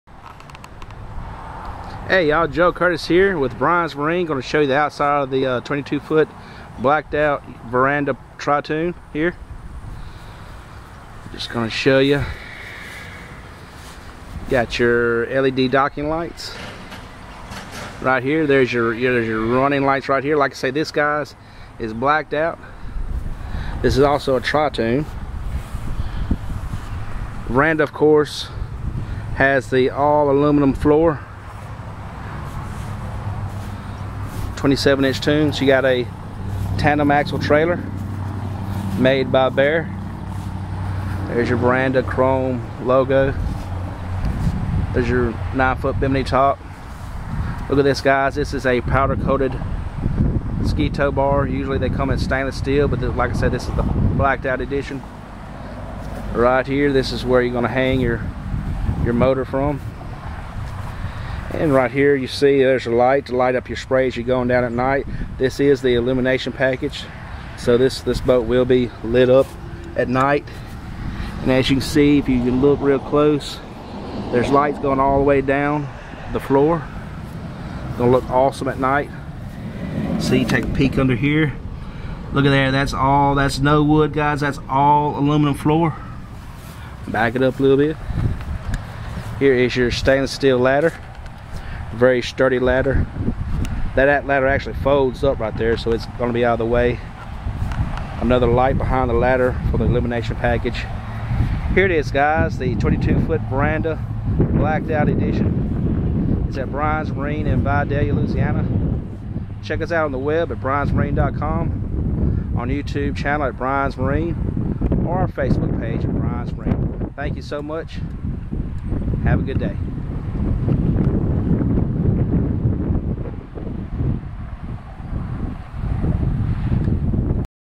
2018 22' Blacked Out Veranda
Look what we have at Bryan's Marine in Vidalia, Louisiana: a 2018 22' blacked out Veranda.